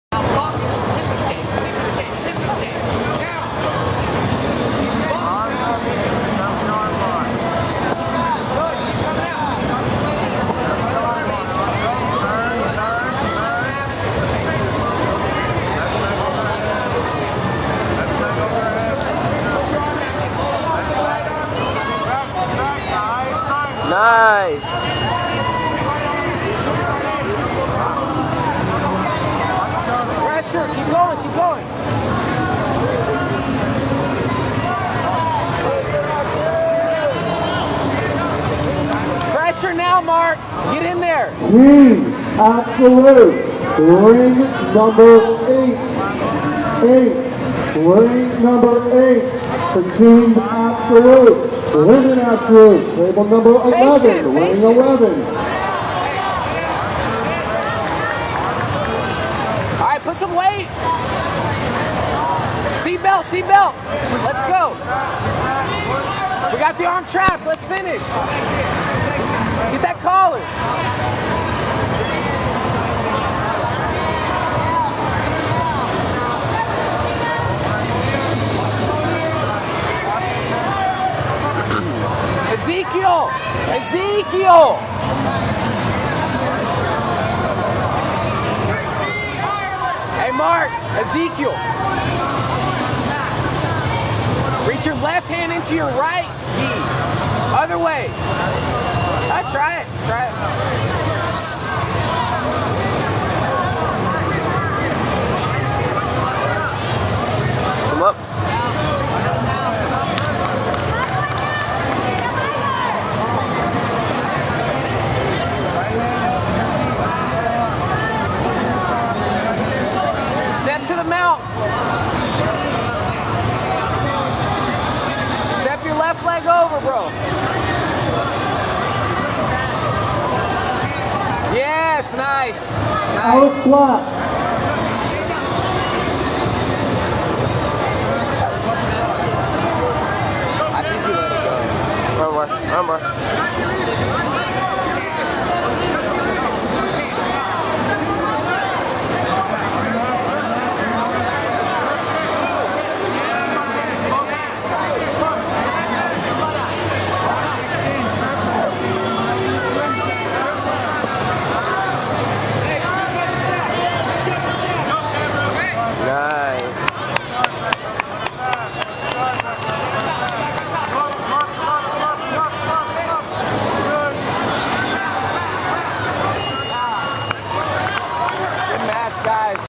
Mark Delarosa Genesis Jiu Jitsu Dallas Europa 2012
Mark in his first match of the day.